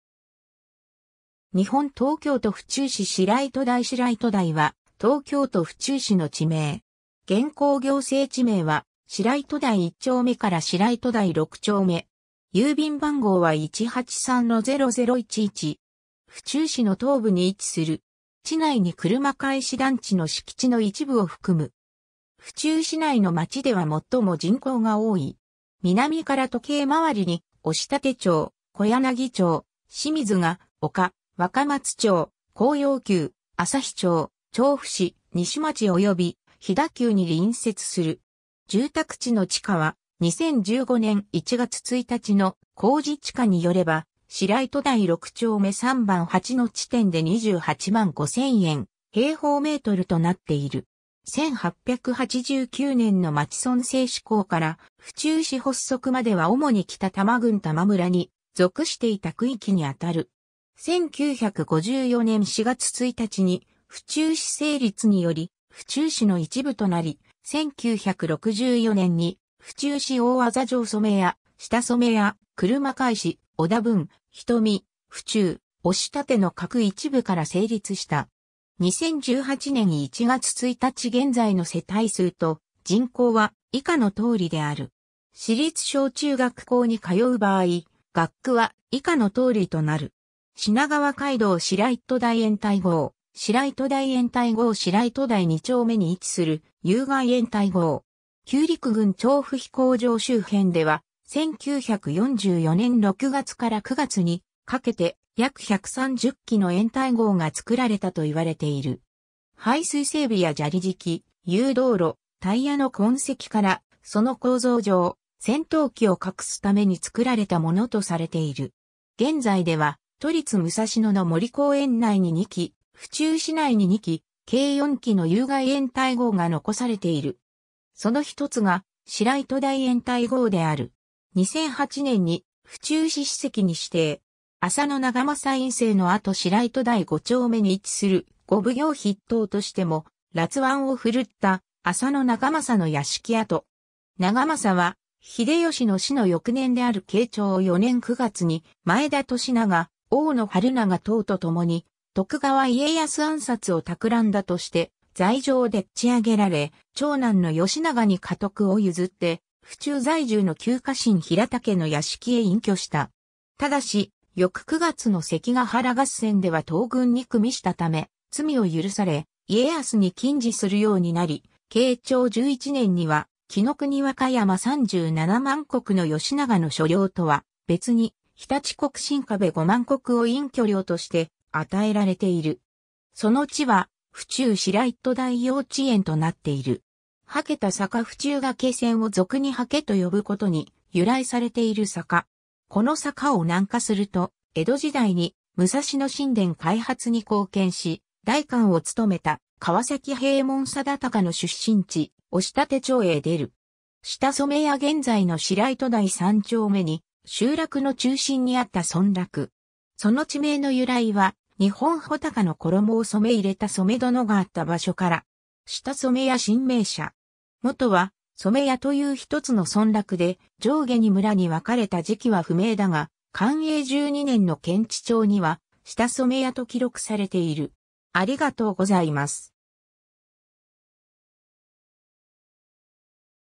白糸台
日本 」 東京都 」 府中市 」 白糸台 白糸台（しらいとだい）は、東京都府中市の地名。
現行行政地名は白糸台一丁目から白糸台六丁目。
郵便番号は183-0011（武蔵府中郵便局管区）。
 府中市の東部に位置する。
地内に車返団地の敷地の一部を含む。
府中市内の町では最も人口が多い。
南から時計回りに押立町、小柳町、清水が丘、若松町、紅葉丘、朝日町、調布市西町および飛田給に隣接する。
 住宅地の地価は、2015年（平成27年）1月1日の公示地価によれば、白糸台6丁目3番8の地点で28万5000円/m2となっている。
 1889年（明治22年） の町村制施行から府中市発足までは主に北多摩郡多磨村に属していた区域に当たる。
1954年（昭和29年）4月1日に府中市成立により府中市の一部となり、1964年（昭和39年）に府中市大字上染屋・下染屋・車返・小田分・人見・府中・押立の各一部から成立した。
 2018年（平成30年）1月1日現在の世帯数と人口は以下の通りである。
 品川街道 白糸台掩体壕 白糸台掩体壕 白糸台二丁目に位置する「有蓋掩体壕」。
旧陸軍調布飛行場周辺では、1944年6月から9月にかけて約130基の掩体壕が作られたと言われている。
排水整備や砂利敷き、誘導路、タイヤの痕跡から、その構造上、戦闘機を隠すために作られたものとされている。
現在では、都立武蔵野の森公園内に2基、府中市内に2基、計4基の有蓋掩体壕が残されている。
その一つが白糸台掩体壕である。
2008年に府中市史跡に指定。
 浅野長政 隠棲の跡 白糸台五丁目に位置する、五奉行筆頭としても辣腕を振るった浅野長政の屋敷跡。
長政は、秀吉の死の翌年である慶長4年9月に前田利長・大野治長等と共に徳川家康暗殺を企んだとして罪状をデッチあげられ、長男の幸長に家督を譲って、府中在住の旧家臣平田家の屋敷へ隠居した。
但し、翌9月の関ヶ原合戦では東軍に組したため、罪を許され、家康に近侍するようになり、慶長11年には紀伊国和歌山370,000石の幸長の所領とは別に常陸国真壁50,000石を隠居料として与えられている。
その地は、府中白糸台幼稚園となっている。
 はけた坂 府中崖線を俗にハケと呼ぶことに由来されている坂。
この坂を南下すると、江戸時代に武蔵野新田開発に貢献し、代官を勤めた川崎平右衛門定孝の出身地、押立町へ出る。
 下染屋 現在の白糸台三丁目に集落の中心にあった村落。
その地名の由来は、日本武尊の衣を染め入れた染殿があった場所から。
下染屋神明社。
もとは「染屋」という一つの村落で、上下二村に分かれた時期は不明だが、寛永十二年（1635）の検地帳には下染屋と記録されている。